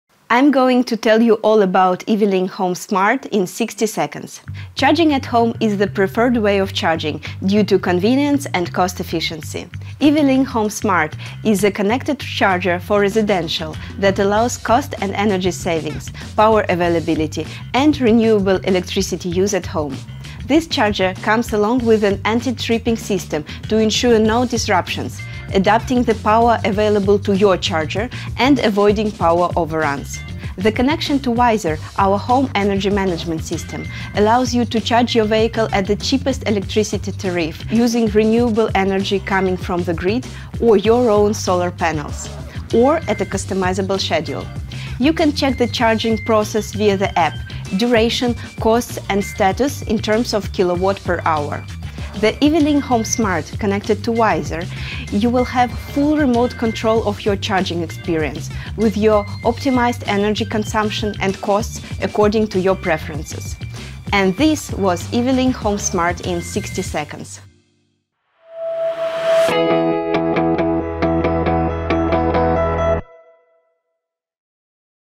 Residential Smart EV Charging | Schneider Charge in 60 Seconds | Schneider Electric
Discover EVlink Home Smart to control your EV charging experience and to create a smart and sustainable energy ecosystem for your home that optimizes savings, comfort, and resiliency. ►Learn more about our home EV charging solution